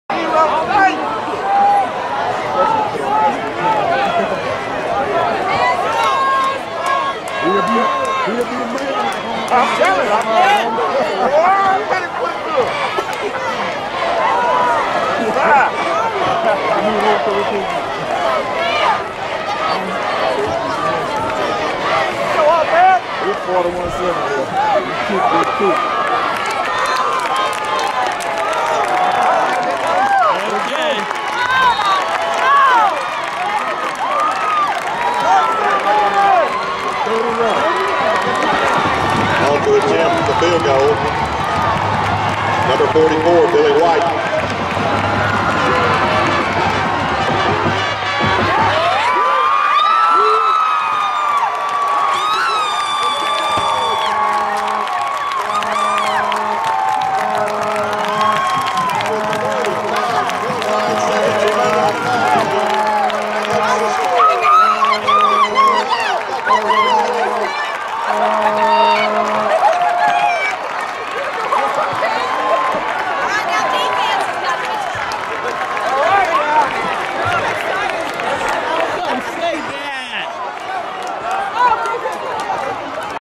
Sparkman Football 2010 Billy Field Goal
Billy Kicks 30 yard field goal with 35 seconds on the clock as Sparkman takes the lead 29-28.